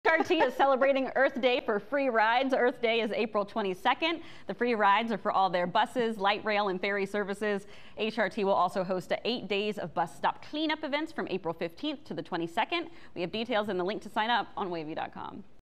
HRT is celebrating Earth Day with free fares
With free fares on April 22, eight days of bus stop cleanups, and ways to save you gas, time and money, Hampton Roads Transit (HRT) and its TRAFFIX program are making it easy to give Mother Earth some love this Earth Day.